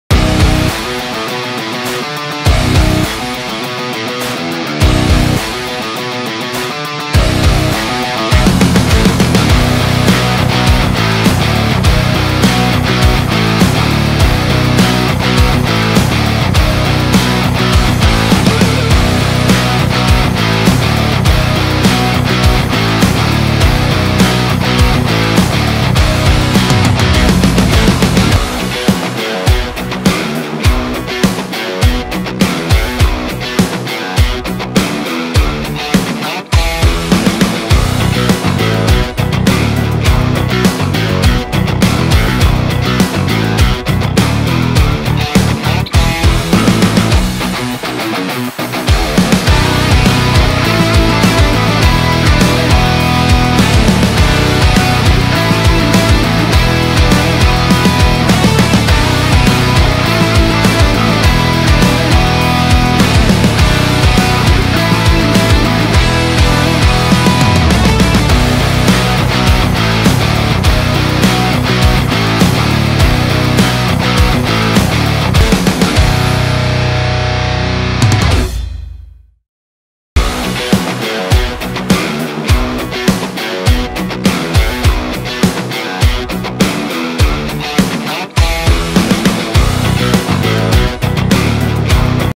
Small Truck, BIG Adventure | LaTrax Teton powered
Don’t let its size fool you, the LaTrax Teton packs full-size RC monster truck fun into a convenient carry-anywhere 1/18 scale size.